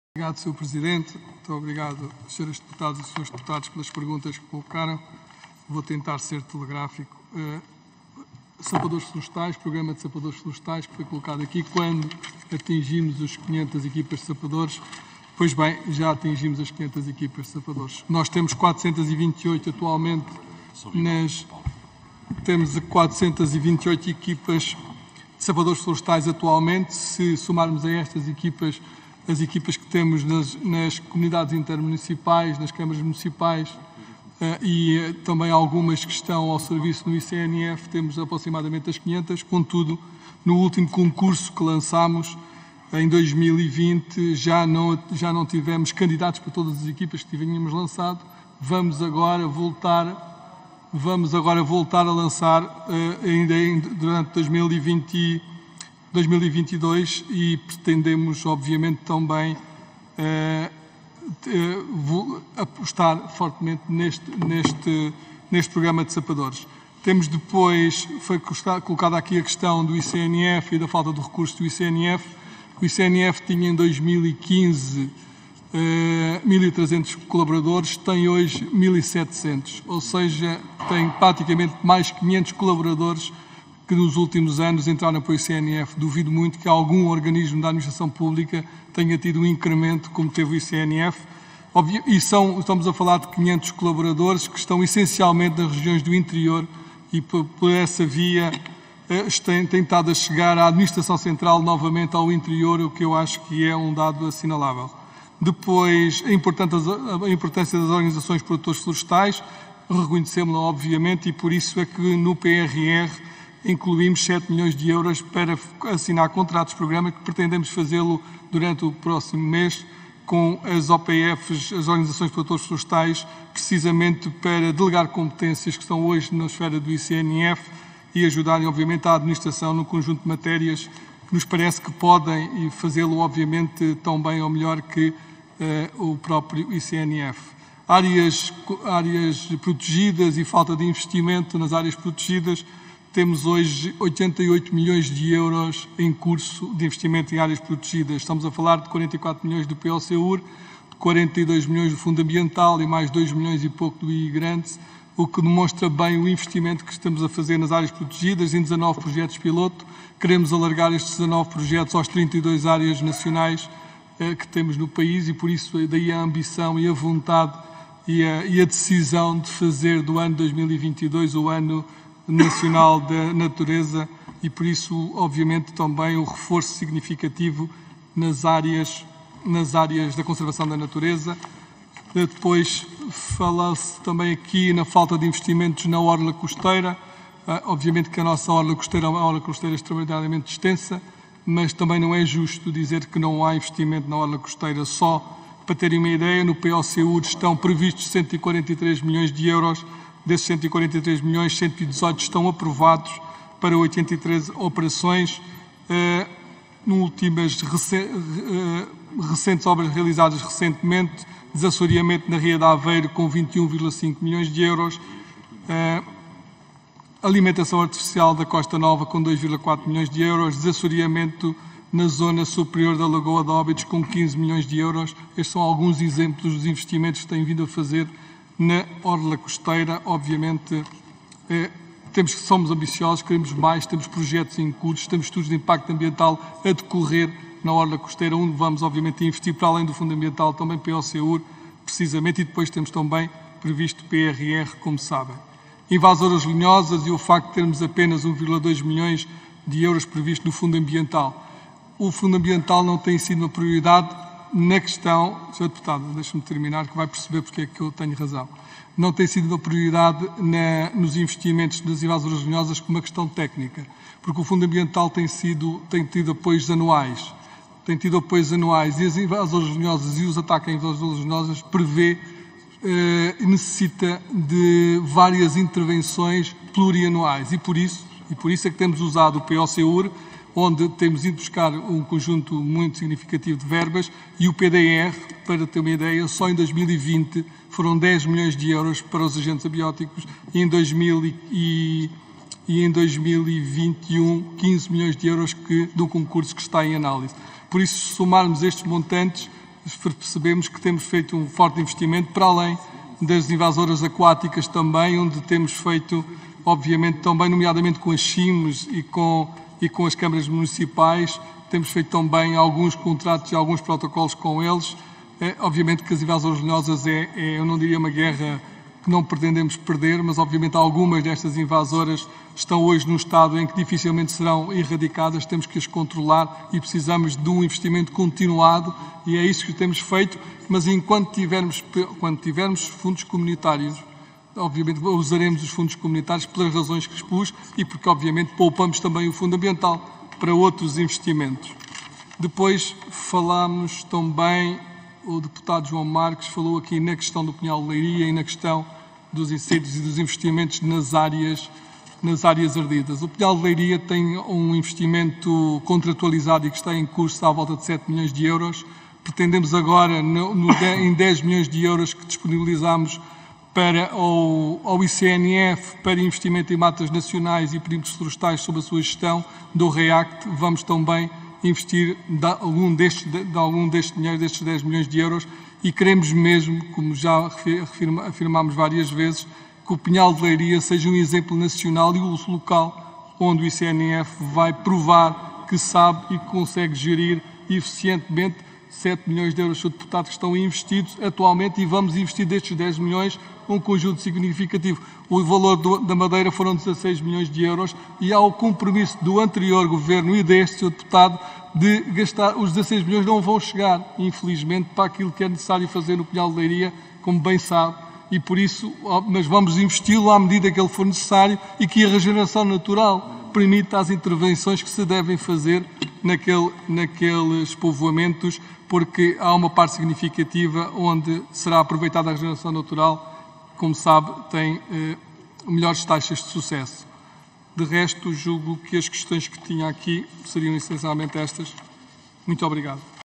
2022 05 11 Resposta do Secretário de Estado da Conservação da Natureza e Florestas, João Paulo Catar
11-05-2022 | Resposta do Secretário de Estado da Conservação da Natureza e Florestas, João Paulo Catarino, a deputados na 2ª ronda, em audição, no âmbito da apreciação, na especialidade, da Proposta de Lei (GOV) - "Aprova o Orçamento do Estado para 2022” - Comissão de Orçamento e Finanças - Comissão de Economia, Obras Públicas, Planeamento e Habitação, Agricultura e Pescas - Comissão de Ambiente e Energia